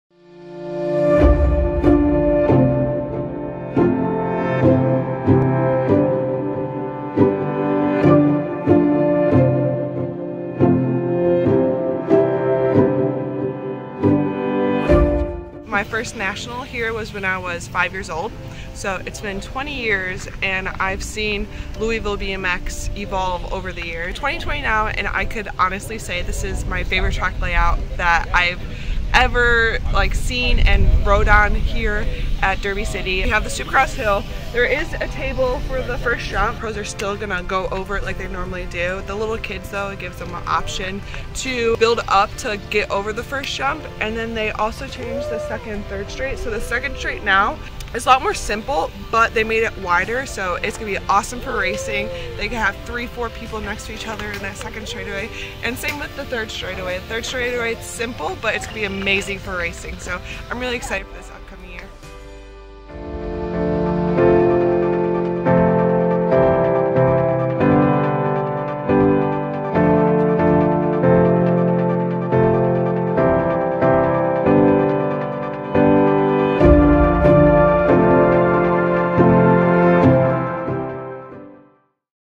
We Are...Derby City BMX!
Felicia Stancil (2019 UCI North American Womens Elite Champion) Talks about Derby City BMX New Rebuild
Season Opener June 17, 2020
Filmed by Rick Carr and Josh Saucier
Edited by Rick Carr
Color Graded by Rick Carr